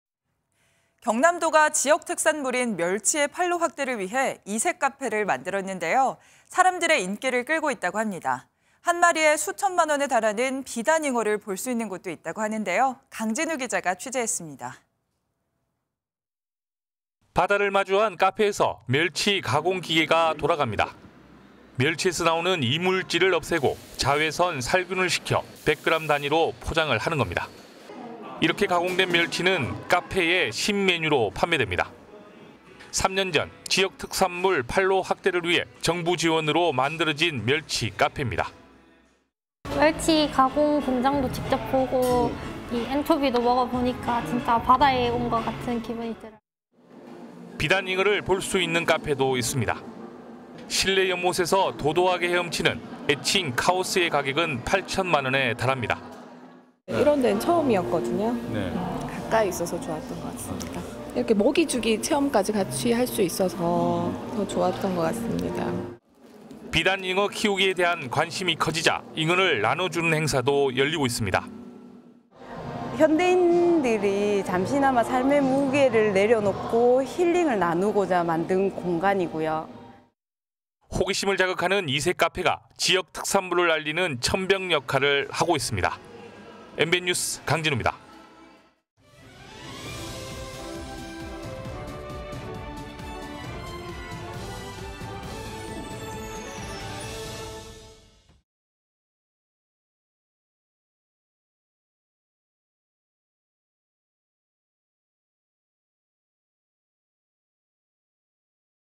"멸치·잉어, 묘하게 어울려"…지역 특산물 알리는 이색카페 [전국네트워크]
【 앵커멘트 】
  경남도가 지역 특산물인 멸치의 판로 확대를 위해 이색 카페를 만들었는데 인기를 끌고 있다고 합니다.
  한 마리에 수천만 원에 달하는 비단잉어를 볼 수 있는 곳도 있다는데요.
  강진우 기자가 취재했습니다.

【 기자 】
  바다를 마주한 카페에서 멸치 가공 기계가 돌아갑니다.

  멸치에서 나오는 이물질을 없애고 자외선 살균을 시켜 100g 단위로 포장을 하는 겁니다.

  이렇게 가공된 멸치는 카페 신메뉴로 판매됩니다.

  3년 전, 지역 특산물 판로 확대를 위해 정부 지원으로 만들어진 멸치 카페입니다.

▶ 인터뷰 : 박수아 / 경남 거제 아주동
- "멸치 가공 공장도 직접 보고 앤초비도 먹어보니까 진짜 바다에 온 것 같은…."

  비단잉어를 볼 수 있는 카페도 있습니다.

  실내 연못에서 도도하게 헤엄치는 애칭 '카오스'의 가격은 8천만 원에 달합니다.

▶ 인터뷰 : 김주희 / 경남 김해 장유면
- "이런 데는 처음이에요. 집 가까이 있어서 좋고 먹이주기 체험까지 할 수 있어서 더 좋은 것 같아요."

  비단잉어 키우기에 대한 관심이 커지자 잉어를 나눠주는 행사도 열리고 있습니다.

▶ 인터뷰 : 조영미 / 비단잉어 카페 대표
- "현대인들이 잠시나마 삶의 무게를 내려놓고 힐링을 나누고자 만든 공간이고요."

  호기심을 자극하는 이색 카페가 지역 특산물을 알리는 첨병 역할을 하고 있습니다.

  MBN뉴스 강진우입니다.

  영상취재: 진은석 기자